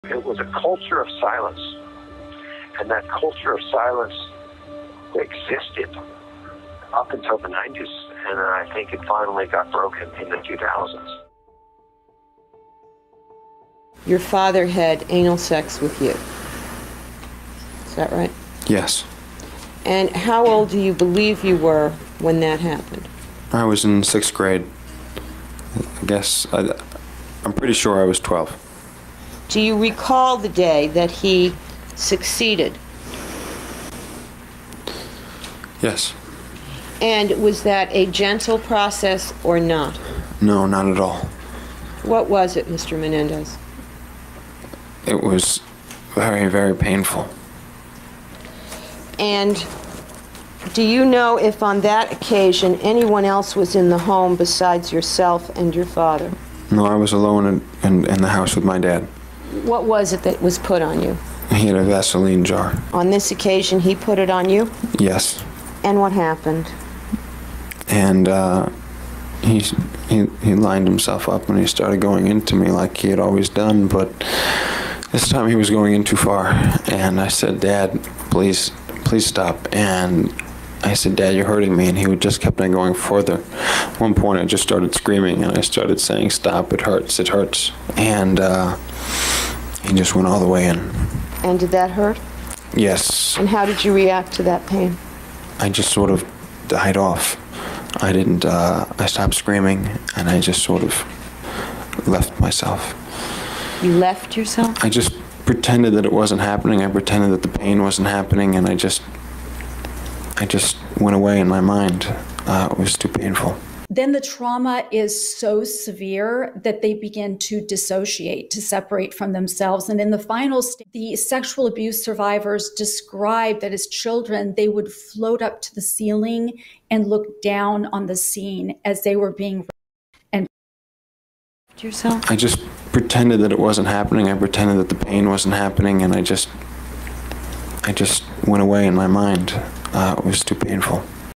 Erik Menendez Testifies He “Left Himself” When His Father Abused Him
Erik Menendez testifies how he handled the painful abuse of his father, Jose Menendez at his trial.